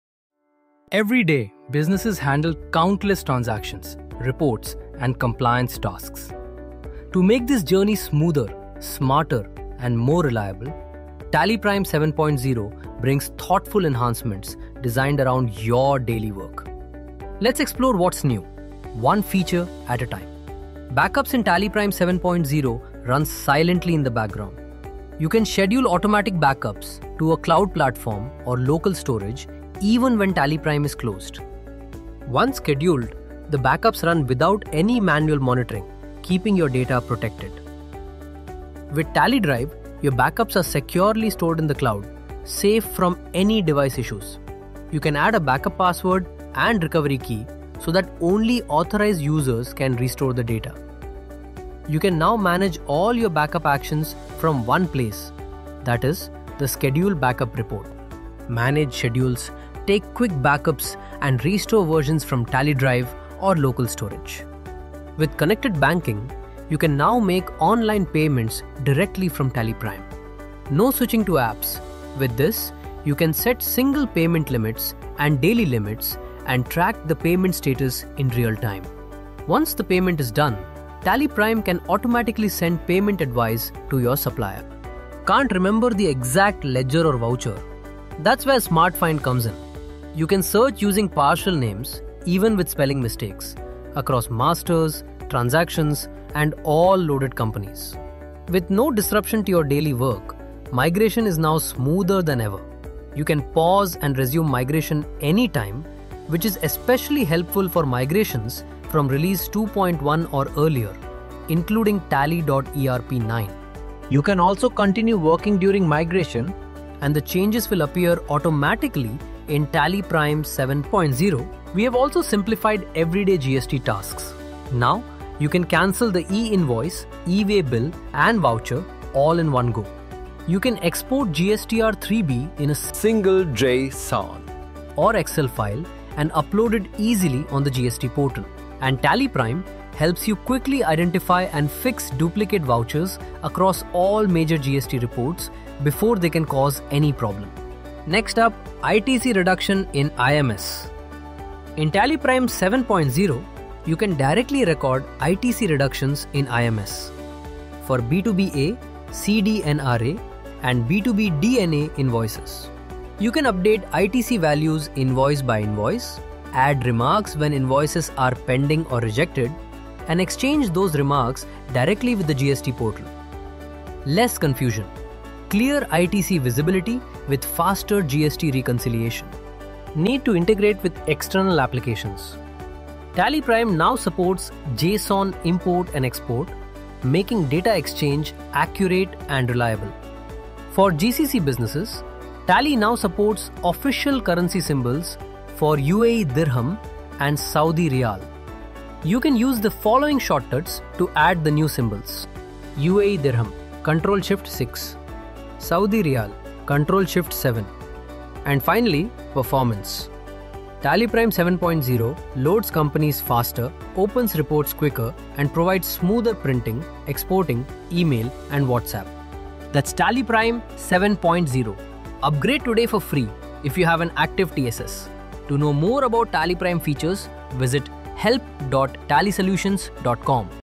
What's New in TallyPrime Release 7.0?
Learn about the latest offerings in TallyPrime Release 7.0.

Migrate from TallyPrime 2.1 or earlier or Tally.ERP 9 5.0 or Later to the Latest TallyPrime Release